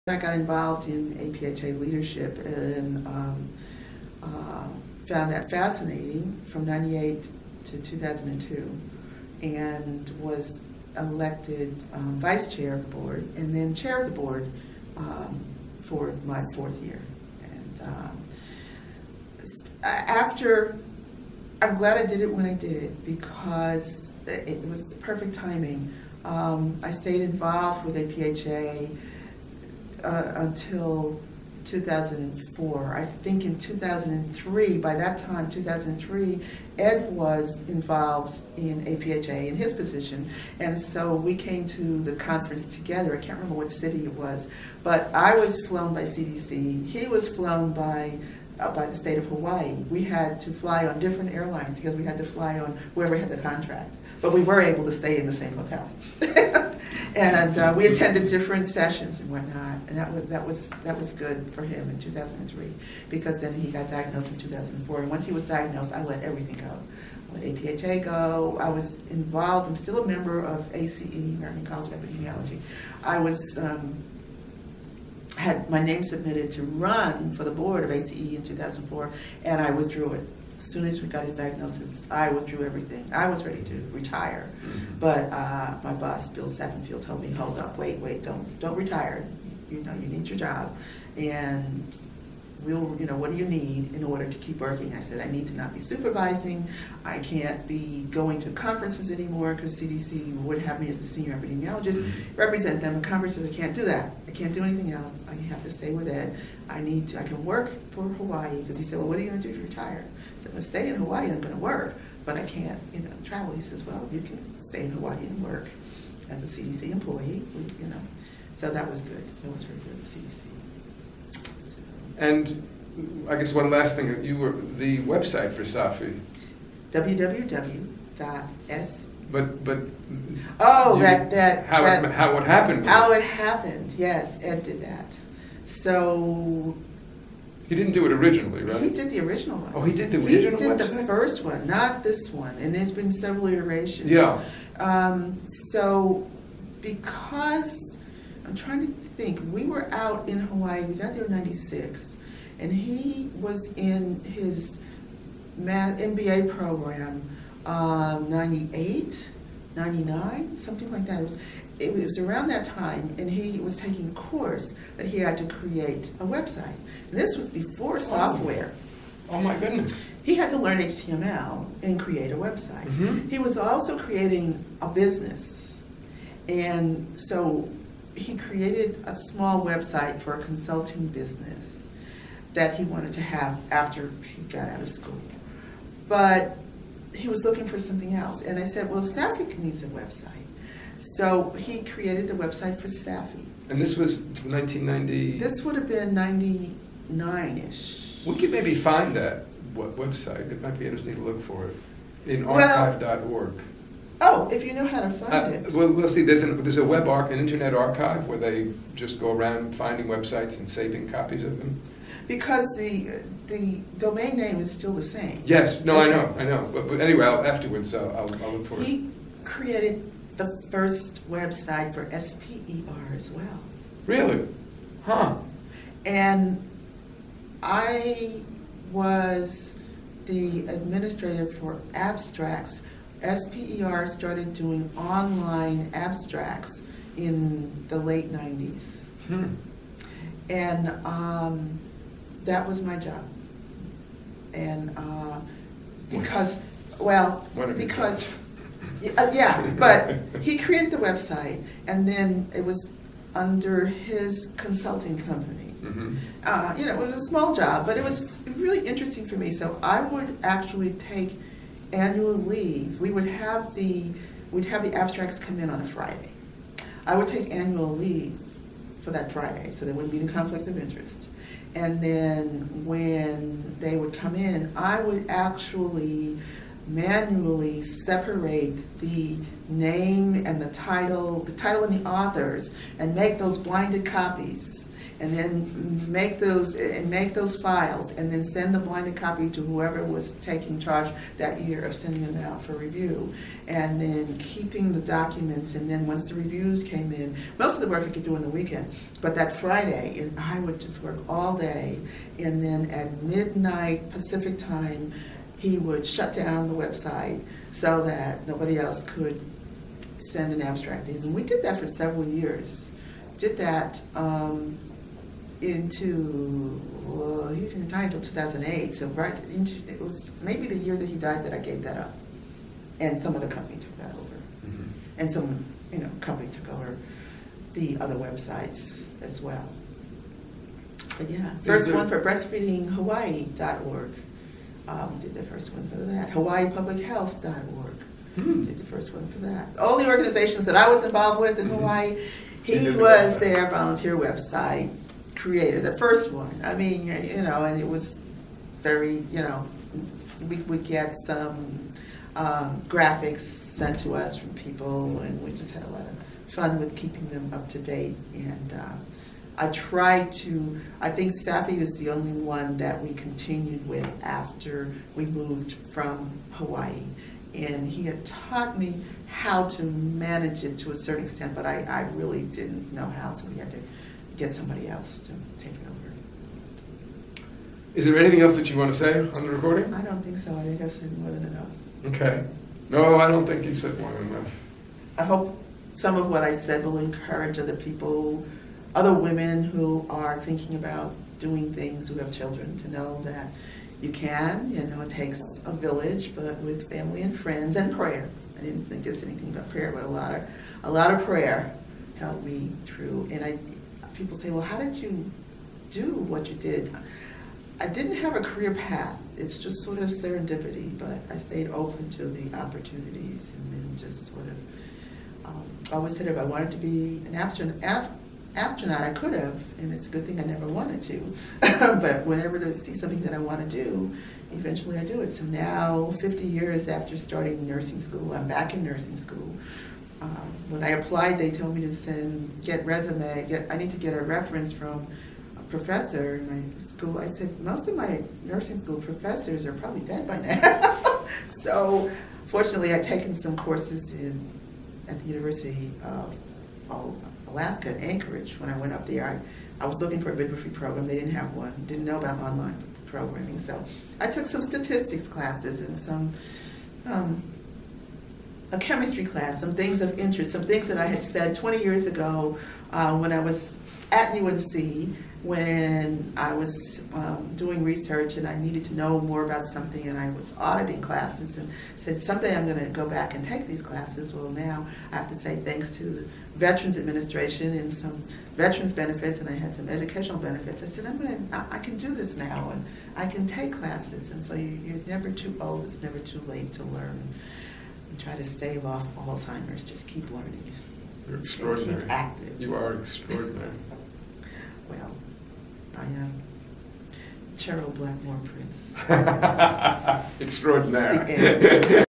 Cheryl Blackmore Prince interview, 11/7/2017, part 4 / 4
Victor Schoenbach interviewed Cheryl Blackmore Prince about her youth; her education at SUNY Buffalo, University of Colorado Medical Center, Emory University, UNC at Chapel Hill, University of Alaska at Anchorage, and Georgetown University; her career as a nurse, EIS Officer, CDC training coordinator, faculty member, and epidemiologist; and her service on the Executive Board of the American Public Health Association (APHA) and as long-
time treasurer of the Society for the Analysis of African American Public Health Issues (SAAPHI). Dr. Prince earned her PhD in epidemiology at the UNC Gillings School of Global Public Health in 1992.
The interview took place November 7, 2017 at the Georgia World Congress Center during the 2017 APHA Annual meeting and was recorded by Vic Schoenbach. Part 4 of 4.